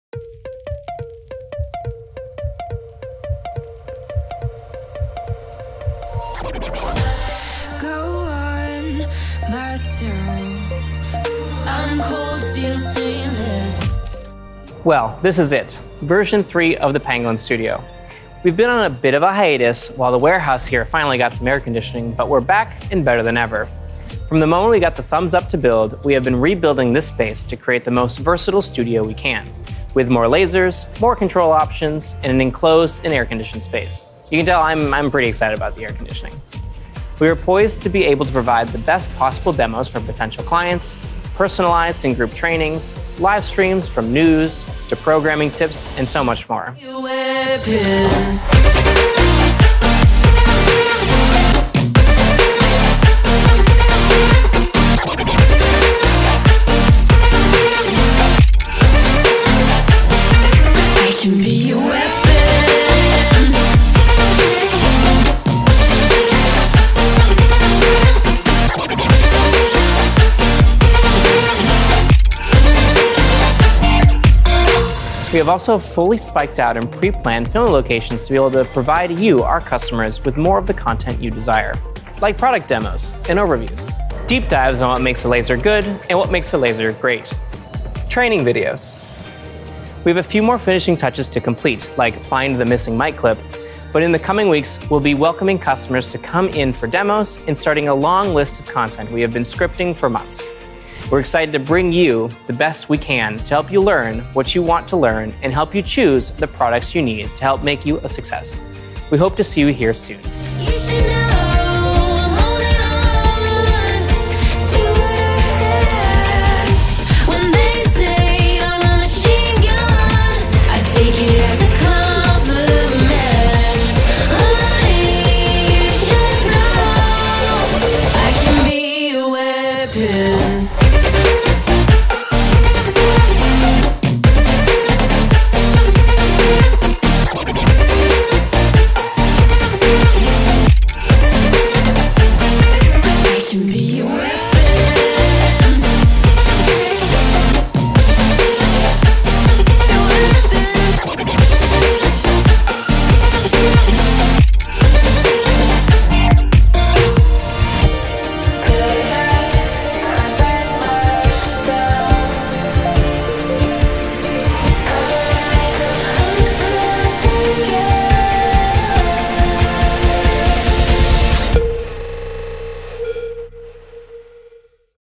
The Pangolin Studio Version #3
Version 3 of the Pangolin Studio is finally here, with an arsenal of more lasers, more control options, and even air-conditioning.

A huge shoutout to our incredible team that took the time to build and bring this dream studio to life.  Their dedication and efforts have created an amazing space to let creativity run with lasers.

With the new studio, we're looking forward to providing more live streams, tips and tricks, product demonstrations, and laser training!